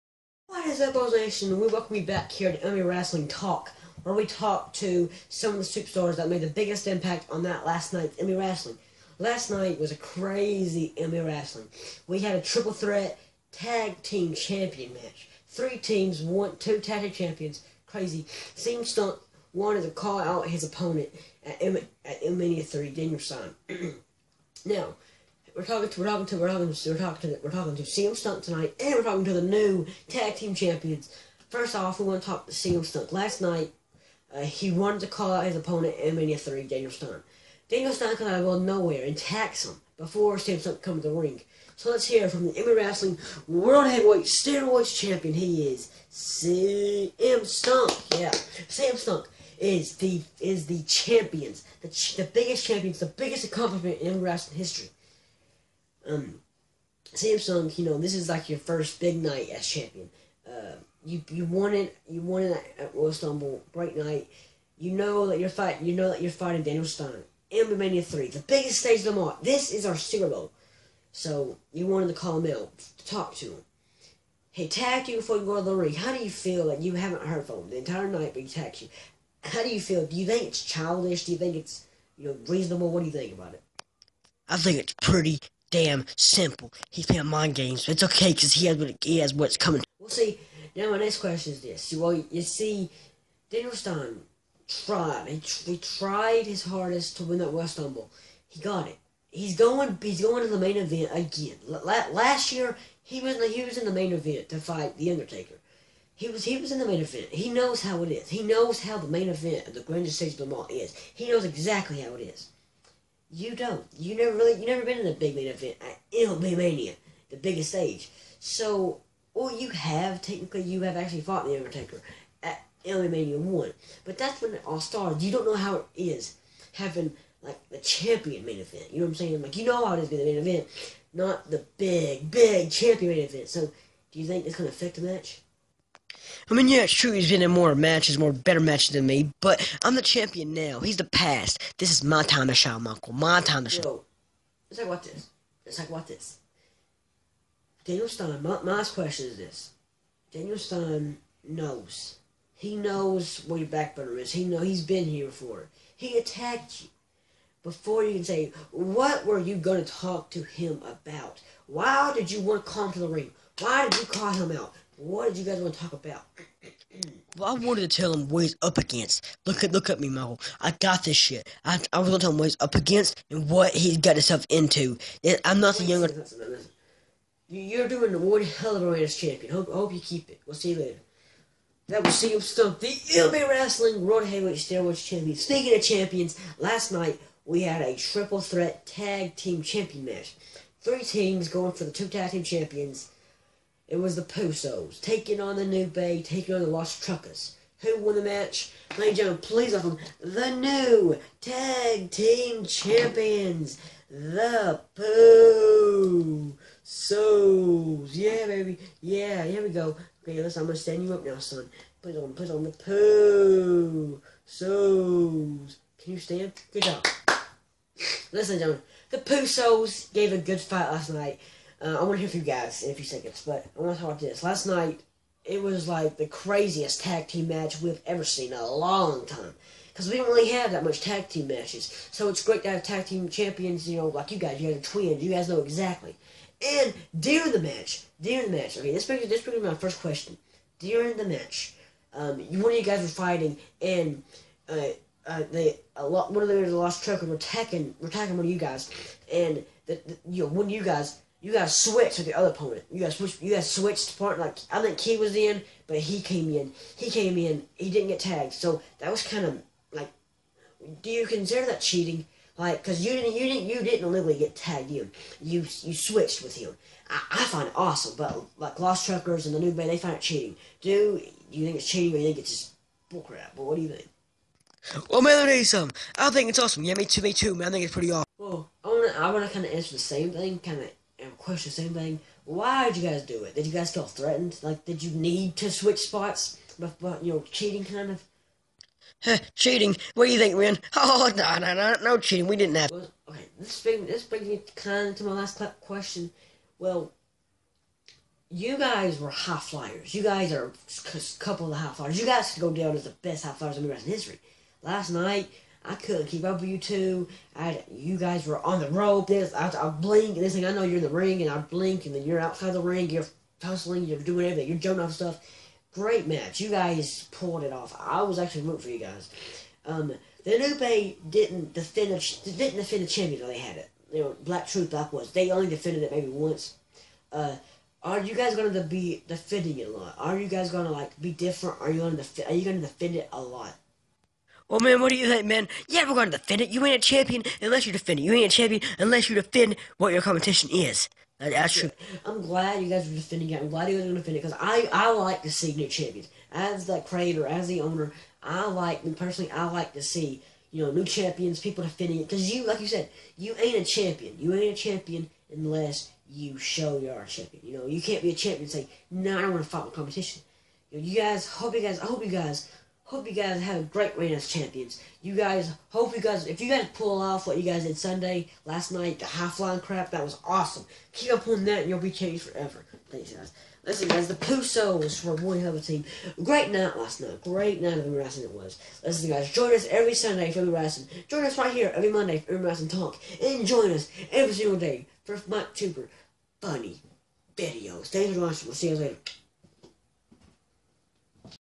Mb Wrestling Talk Ep23!
The new Tag team Champions are on Today On Mb Wrestling talk And So Is the MB wrestling World Champion Cm stunk! What Will Stunk have To say since what Happened Last night with Daniel Attacking Him?

our website and media